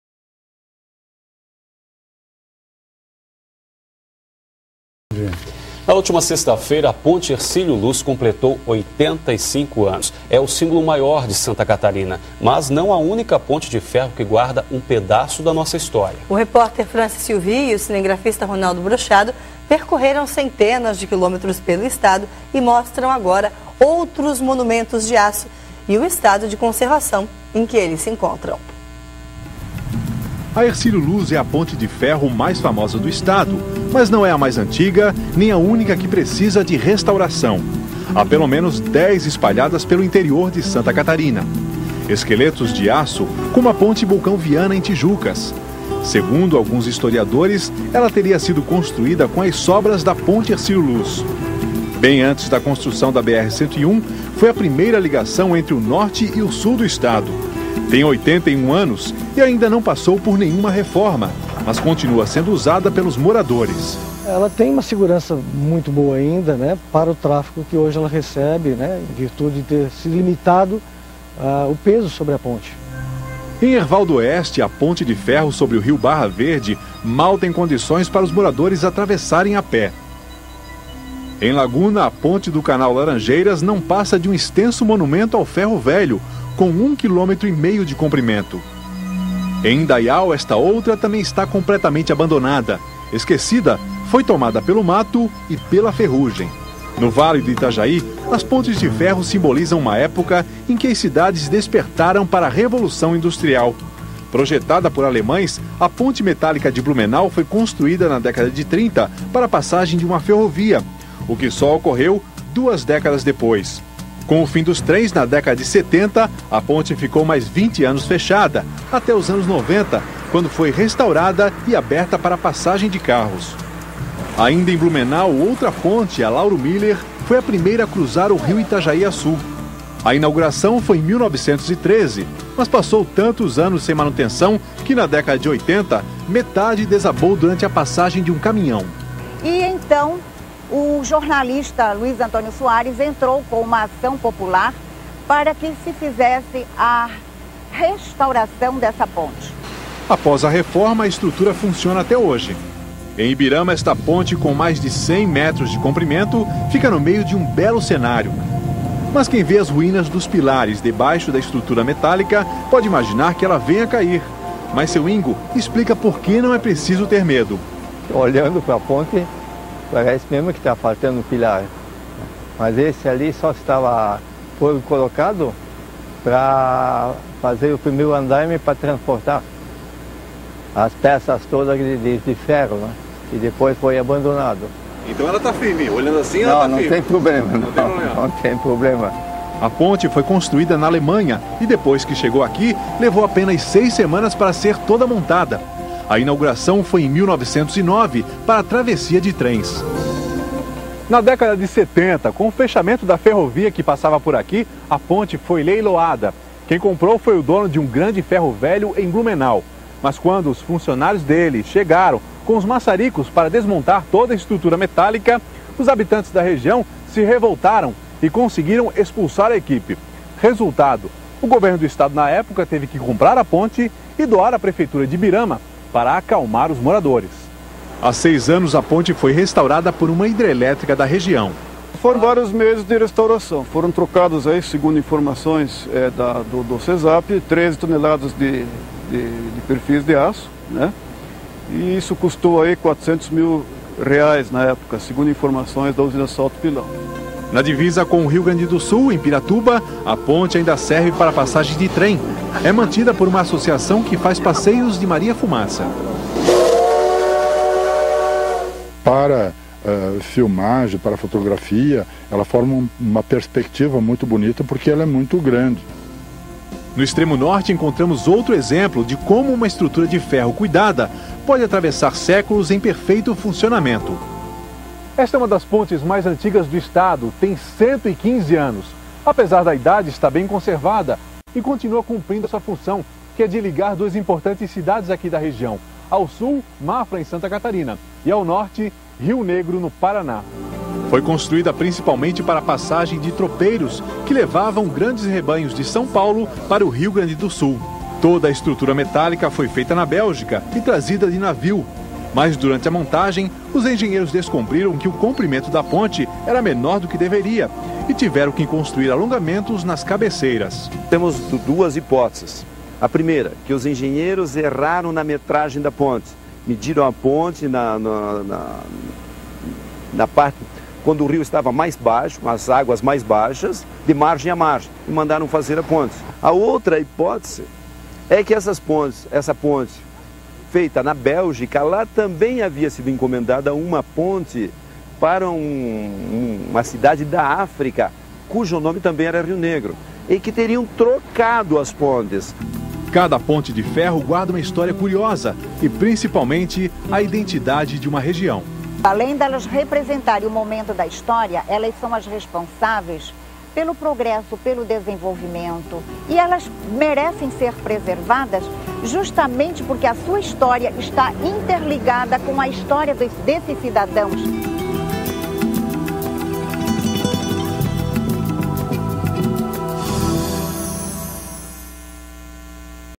As pontes de Ferro em Santa Catarina - (Brasil)
Reportagem exibida pela RBS TV de Santa Catarina sobre pontes de ferro.
A maioria das citadas são ou foram para uso ferroviário.

16/05/2011